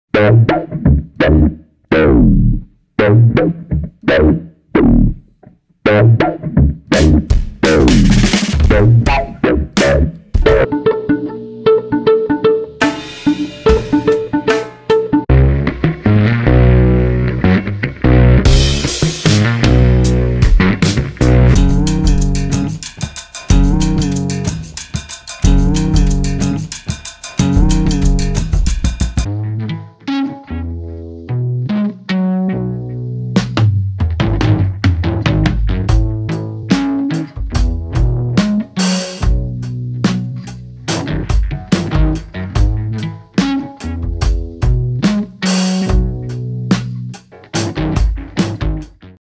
Gabe Noel (Kendrick Lamar, Vulfpeck) Cello Vol1
This is not the cello library you were probably expecting. Melodic and moving? Yes. Clean and classical? Definitely not.

Introducing Gabe Noel (Kendrick Lamar, Father John Misty, Vulfpeck) Cello Vol 1. Heavily inspired by Arthur Russel's avant-garde approach to the instrument, Gabe Noel opens up a vast world of sonic and harmonic pathways throughout this collection.